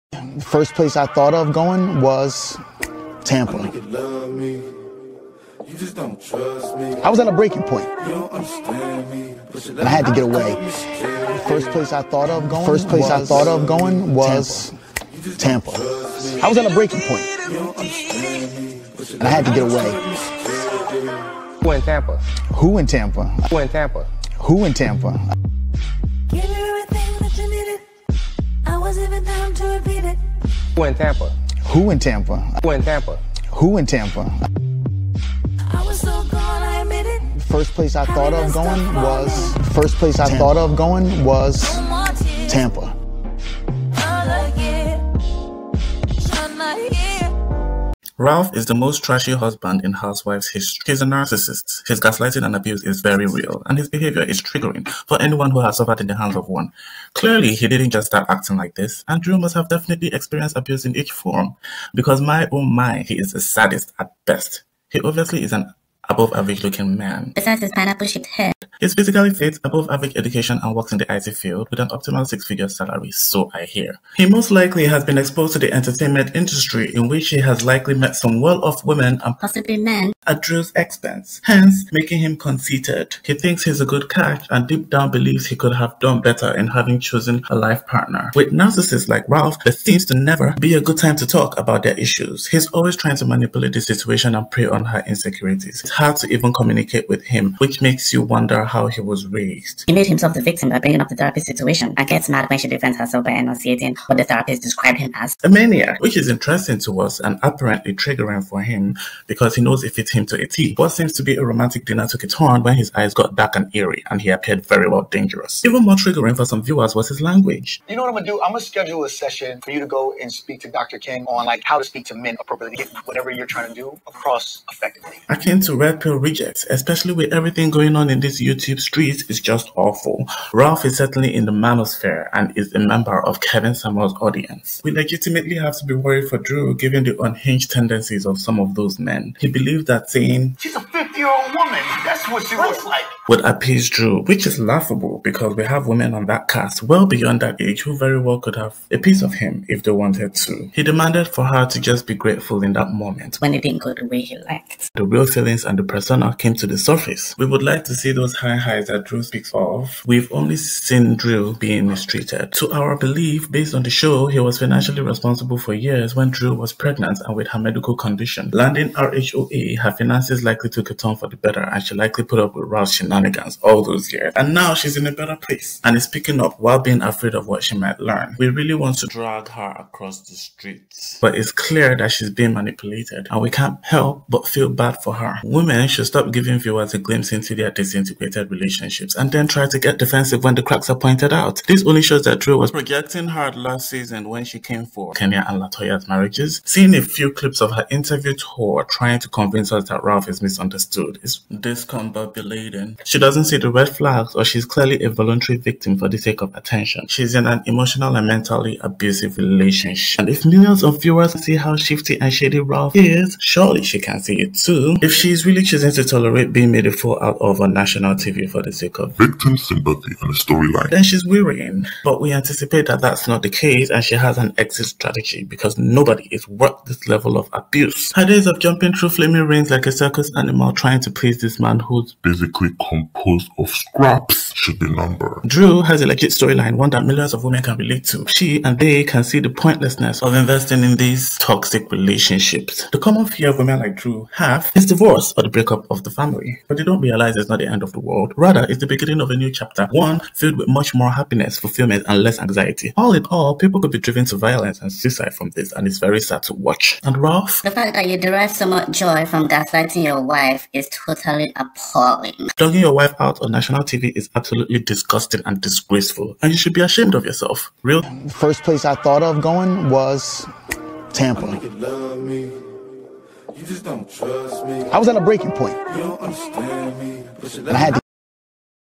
All about Ralph
RHOA s14
Another episode of Yes! to the rehearsed reads
Ralph in the hot seat this time

(p/s:  none of what I say is factual, all alleged, and based off episodes and events as graphic as they come all done in humor )